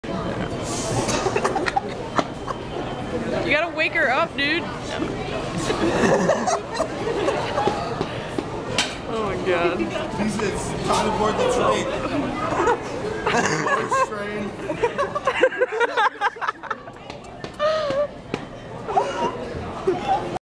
george train
george and i went to claires and got him some fake diamond earrings while all of us waited at the train station. then it was suggested that he popped his collar, which then led to him gelling his hair back. unfortunately for lisa this resulted in a terrible wakeup call. "wake up lisa... it's time to ride the train.. THE GEORGE TRAIN!"